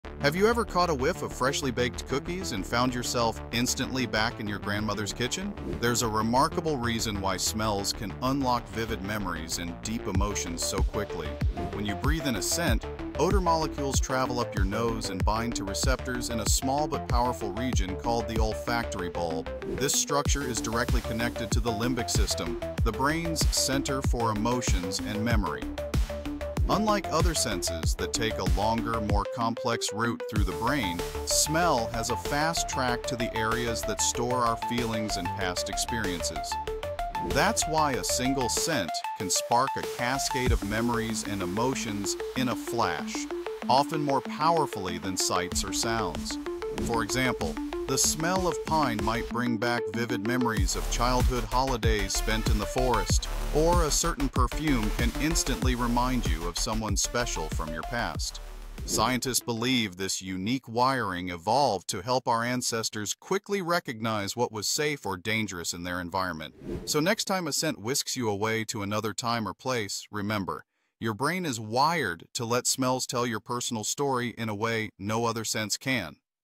How Scents Unlock Hidden Memories Instantly
Ever wondered why a single whiff can transport you to childhood? Discover the science behind scent and memory!
🔥 Welcome to MindBlowingFax — where your brain gets a daily upgrade!
🤯 Wild facts you won’t believe — all in under 90 seconds
🧠 From the weird to the "how is that even real?" — we’ve got it all